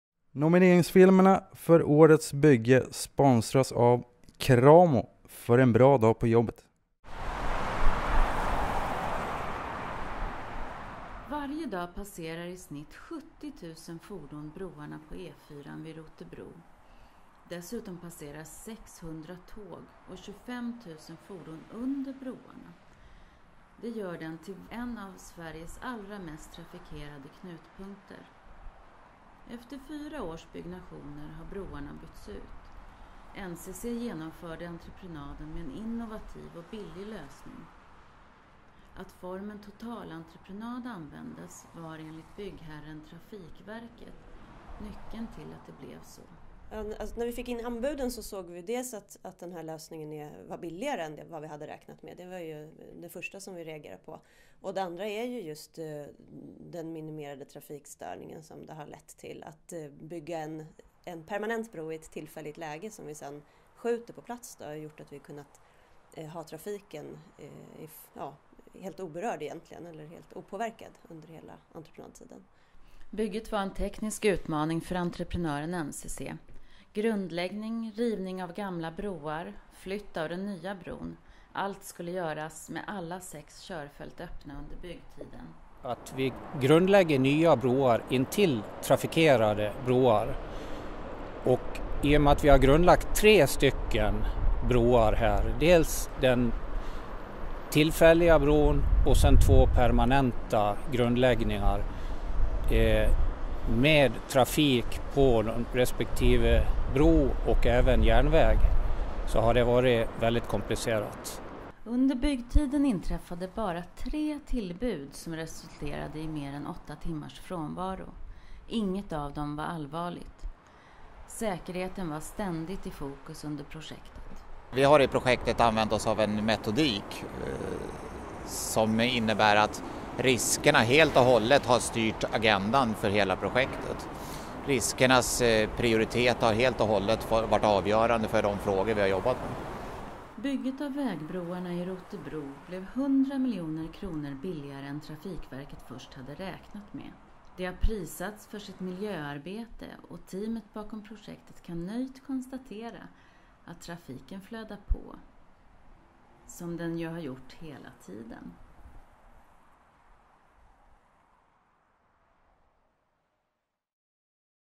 Rotebrobroarna - Nominerad 4 Årets Bygge 2016
Nomineringsfilm om byggprojektet Rotebrobroarna i Stockholm.
Trafikverkets nya strategi för upphandling öppnade för nytänkande hos entreprenören och möjliggjorde en billigare, effektivare och miljövänligare lösning för vägbroarna över Ostkustbanan på E4:an i Rotebro. Nu är vägbroarna i Rotebro nominerade till Årets Bygge 2016.
Medverkande:
Anna-Maria Erlandsson, Projektledare på Trafikverket
Jan Fernö, projektchef på NCC
Joachim Sävenlid, biträdande projektledare på Trafikverket
Reporter: Nomi Melin Lundgren
Foto/redigering: Mattias de Frumerie